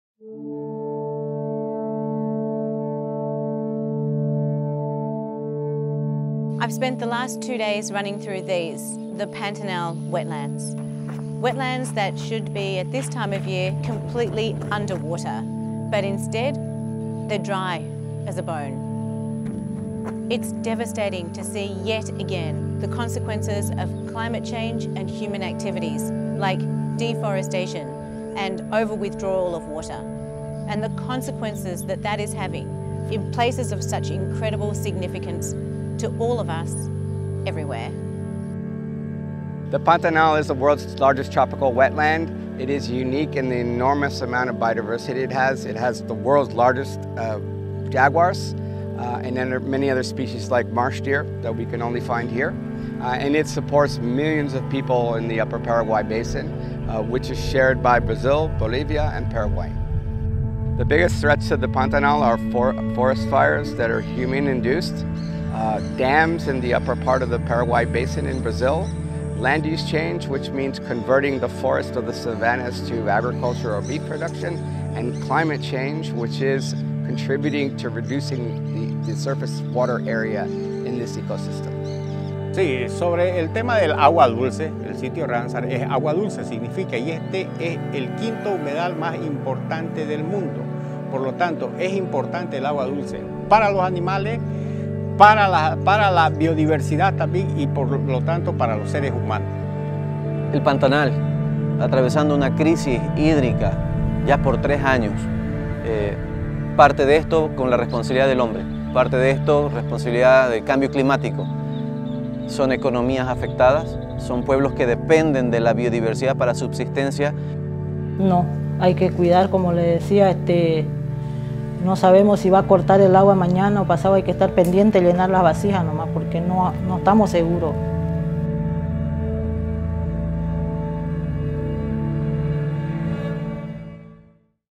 Team RunBlue visited the largest Wetlands in the world - The Pantanal.
We visited the world's largest tropical wetland area. I spent two days running on grounds that are meant to be wet and underwater. But it was not. It was dry as a bone. The impact of ClimateChange and human activity like deforestation and over withdrawal of water in the area is devastating.

The ecosystems and biodiversity that rely on the wetlands are impacted severely.

We need to do more to protect our natural water resources!